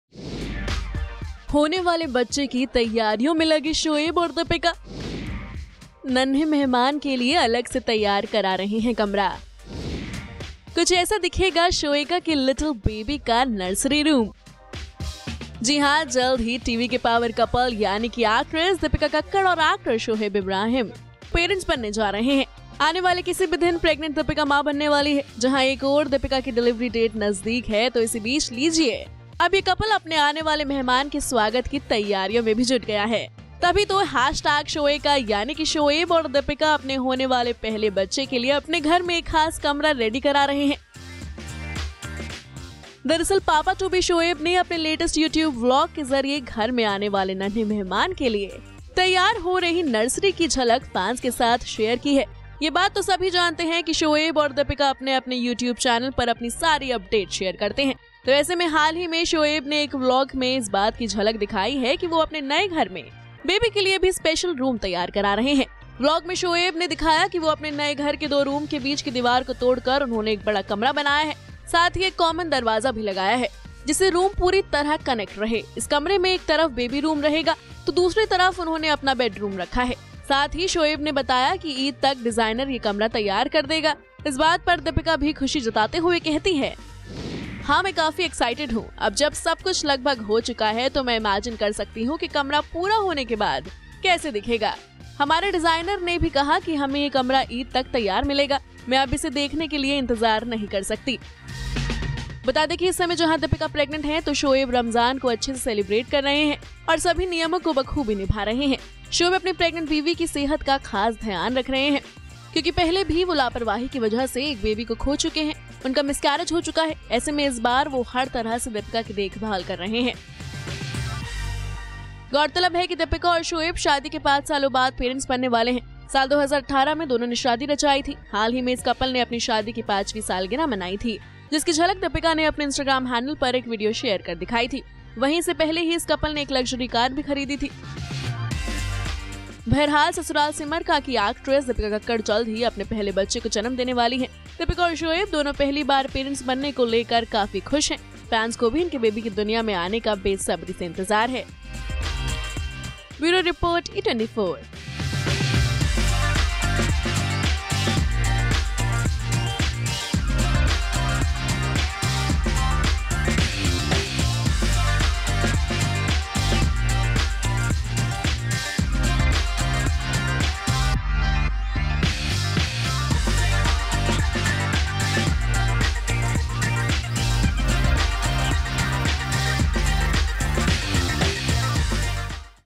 किसी भी वक़्त Dipika Kakkar दे सकती हैं अपने बच्चे को जन्म, नन्हें मेहमान के लिए घर में बनवा रहे कमरा
जल्द ही टीवी के पॉवर कपल यानि कि एक्ट्रेस दीपिका कक्कड़ और एक्टर शोएब इब्राहिम पेरेंट्स बनने जा रहे हैं..आने वाले किसी भी दिन प्रेग्नेंट दीपिका मां बनने वाली हैं..जहां एक और दीपिका की डिलीवरी डेट नजदीक है..तो इसी बीच, लीजिये अब ये कपल अपने आने वाले मेहमान के स्वागत की तैयारियों में भी जुट गया है...तभी तो हैशटैग शोएका यानि कि शोएब और दीपिका अपने होने वाले पहले बच्चे के लिए अपने घर में एक खास कमरा रेडी करा रहे हैं..